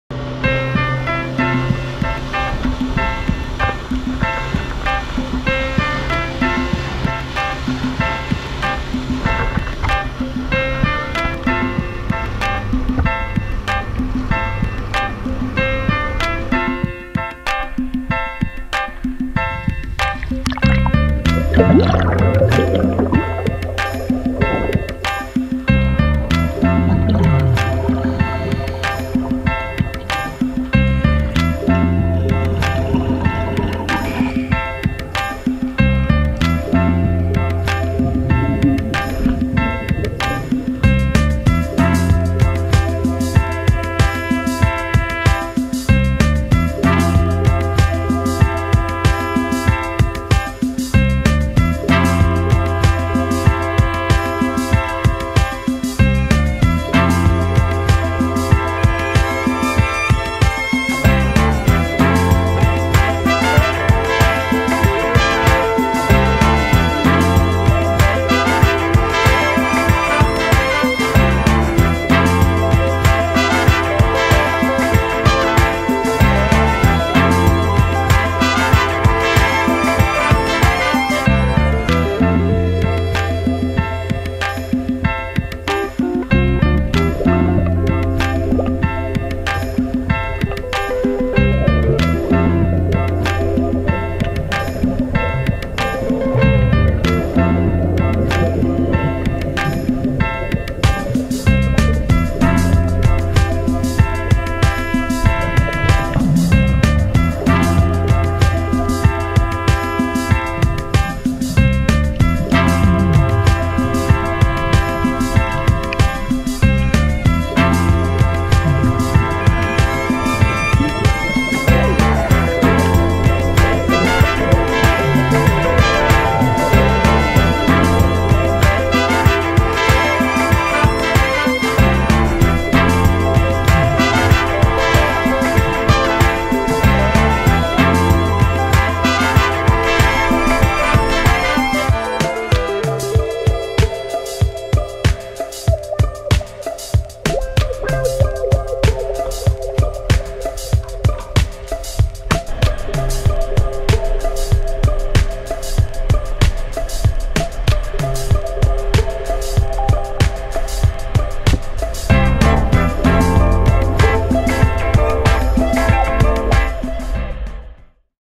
Amazing Scuba Dive Day - The Poor Knights Islands, New Zealand
Clear blue water scuba diving in winter. At the always stunning Poor Knights Islands. Northland, New Zealand
The light was perfect on a dive site on the west side of the islands called Matt's Crack. Full of little Twospot demoiselle taking shelter.